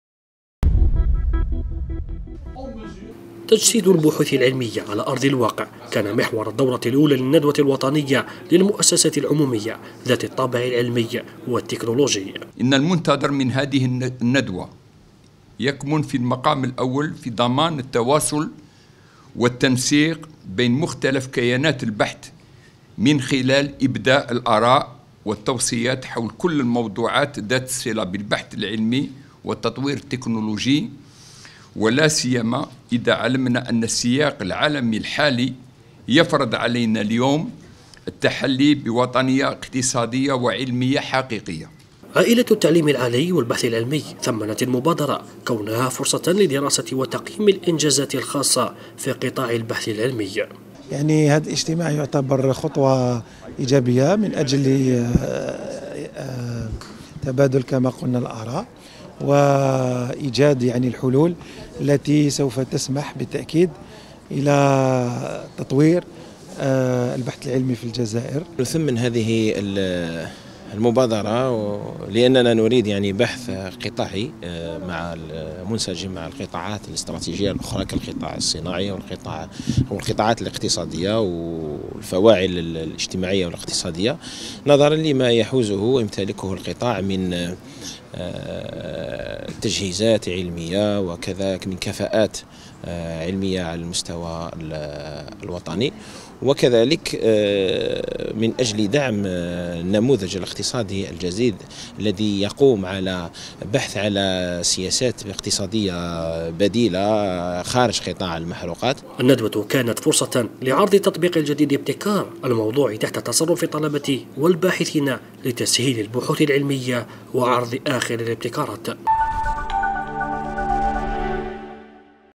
ندوة المؤسسات العمومية العلمية: ضرورة تجسيد البحث العلمي في القطاع الاقتصادي
الجزائر (وأج) - تهدف الندوة الوطنية للمؤسسات العمومية ذات الطابع العلمي والتكنولوجي إلى توطيد الشراكة مع القطاع الاجتماعي والاقتصادي، لاسيما في مجال تثمين نتائج البحث والابتكار والتحويل التكنولوجي وتحديد الطرق والوسائل التي تسمح بتطوير التعاون ما بين مؤسسات البحث الوطنية والدولية.